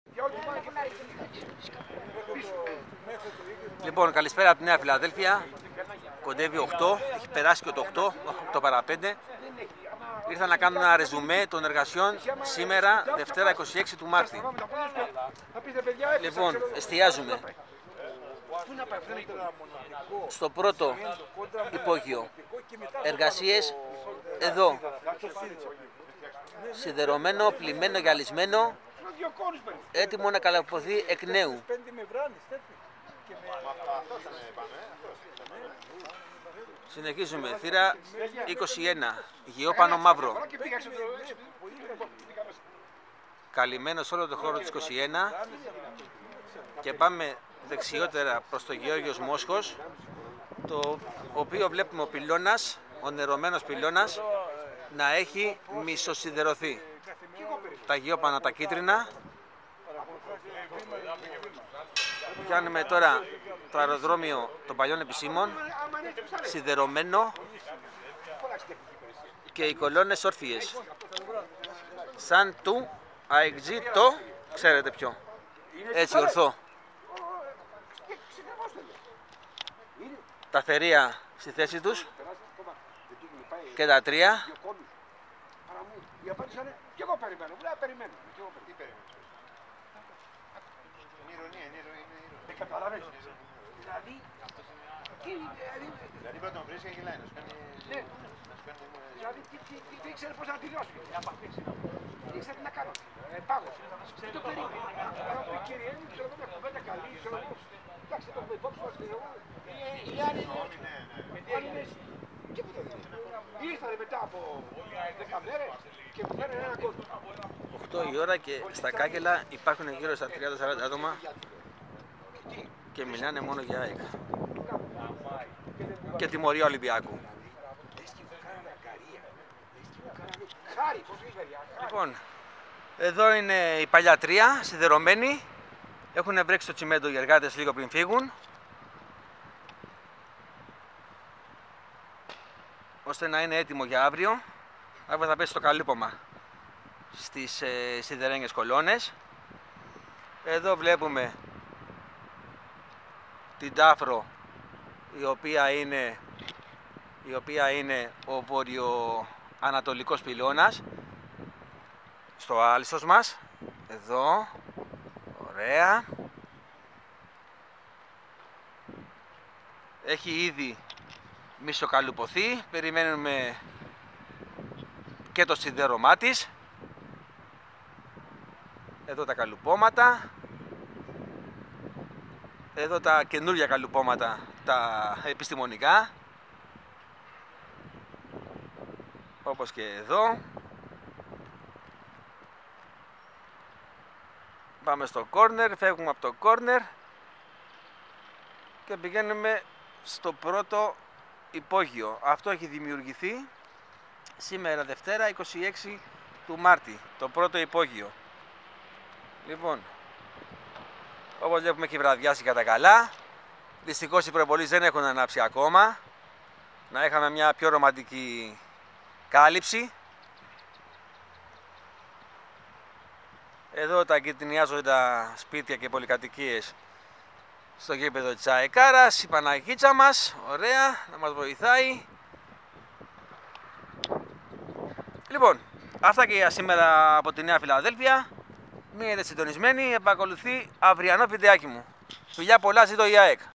ΓΗΠΕΔΟ ΑΕΚ (ΦΟΝΙΑΣ) ΑΓΙΑ ΣΟΦΙΑ!!! ρομαντισμός με νυχτερινή λήψη
Ένα βραδυνό ρεζουμέ από τα τεκτενόμενα στο εργοτάξιο του υπό κατασκευή ναού της ΑΕΚ -  ΑΓΙΑ ΣΟΦΙΑ!!